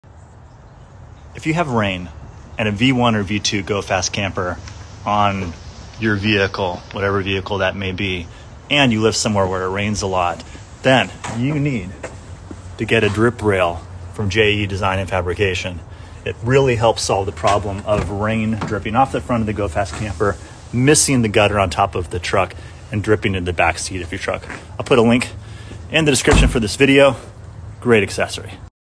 DripRail, gutters for your Go Fast Camper
The DripRail from JE Design and Fabrication directs water from the GoFast Platform Camper to inside the vehicle's factory drip rail, helping keep your backseat dry. Black powder-coated steel. Works on both V1 & V2 campers. Sold as a pair and includes all mounting hardware.